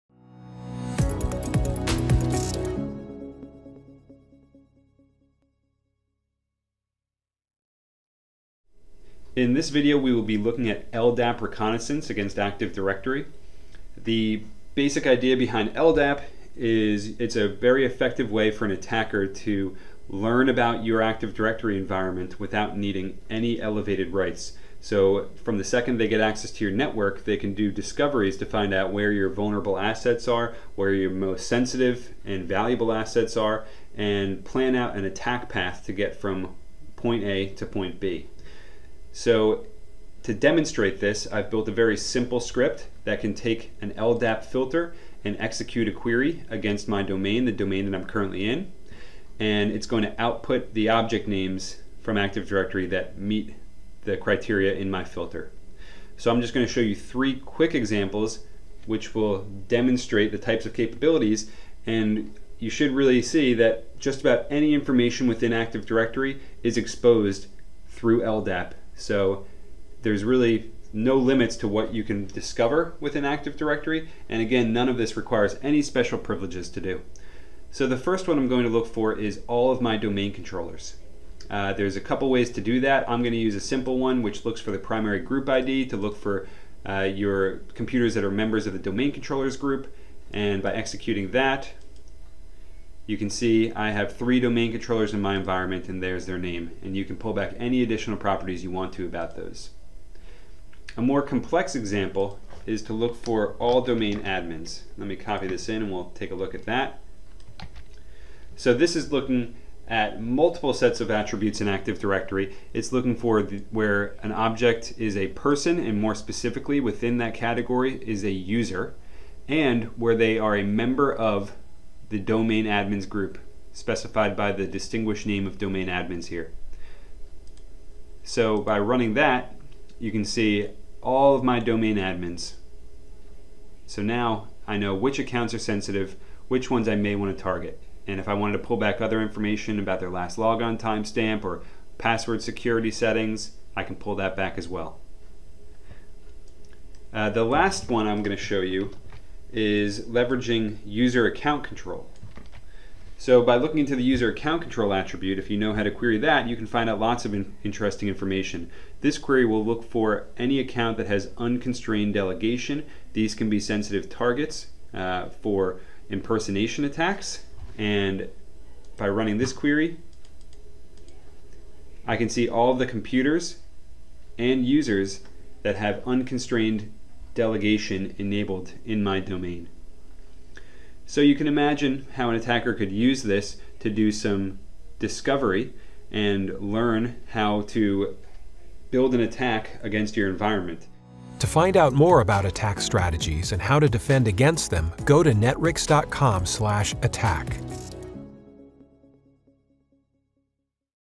Attack Tutorial: Performing Reconnaissance using LDAP
This video demonstrates the basics of Active Directory reconnaissance using LDAP.
When an attacker compromises a system on a network, they initially have little privileges within the domain. However, once they infiltrate any domain-joined computer, they can query the directory and its objects using LDAP to locate sensitive accounts and other assets to target in their attack.
To learn more about LDAP reconnaissance and how to mitigate, detect and respond to it go